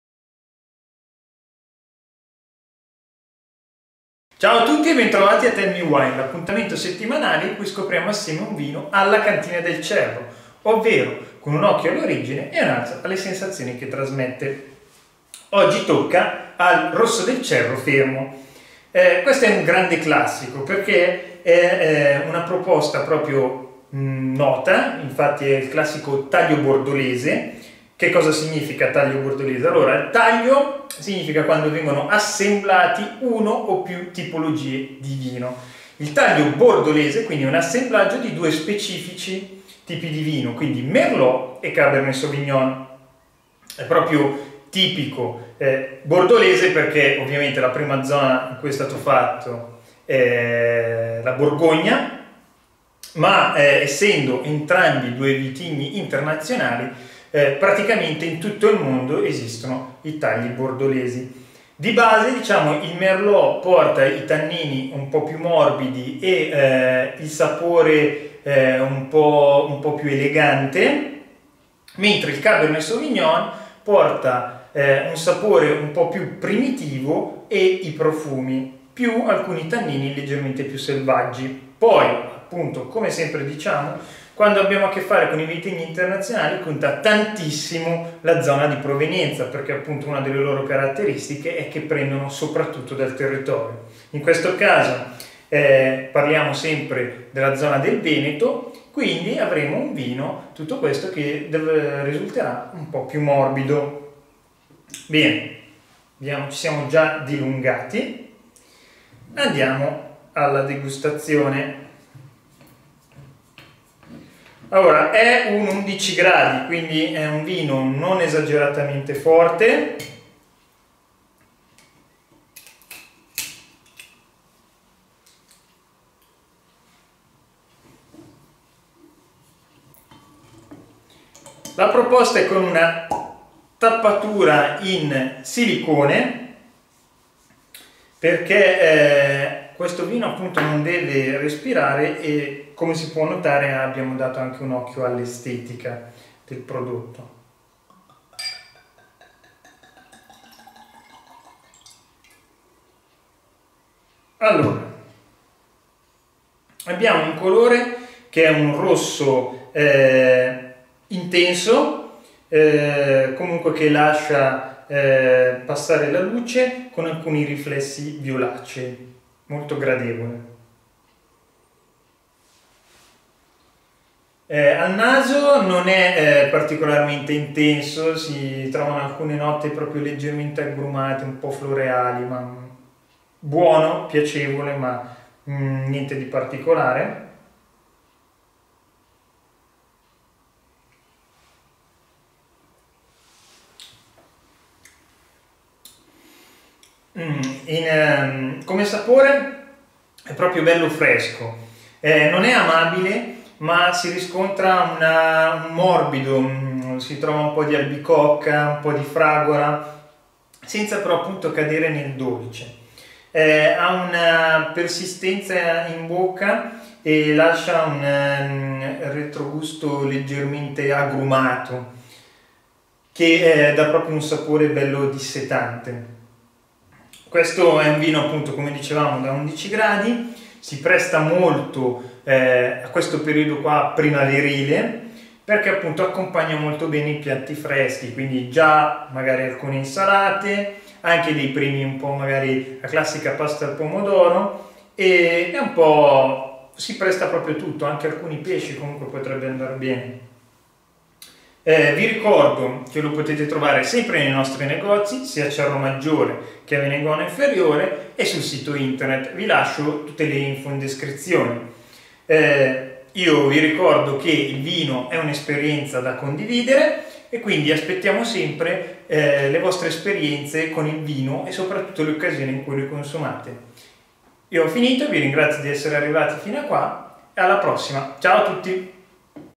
Tell me wine Rosso del Cerro fermo di Cantine del Cerro
Il vino è un'esperienza da condividere!
tellmewine, l'appuntamento settimanale in cui scopriamo insieme un vino... alla cantinedelcerro, con un occhio all'origine e l'altro alle sensazioni che trasmette.
Oggi tocca al Rosso del Cerro di Cantine del Cerro

Per gli ordini mandate un messaggio WhatsApp al numero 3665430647 con nome, cognome e indirizzo